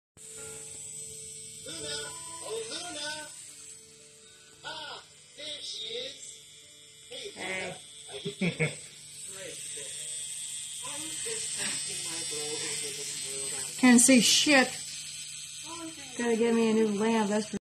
Tattooing on my Boyfriend
I can't see shit. got all the lights I need. Judt tattooing on my sexy thicc marshmallow man. I freaking miss you so much. Please get better soon. I love you with all my heart and soul. boys tattoo tattooartist noob art memories 🙏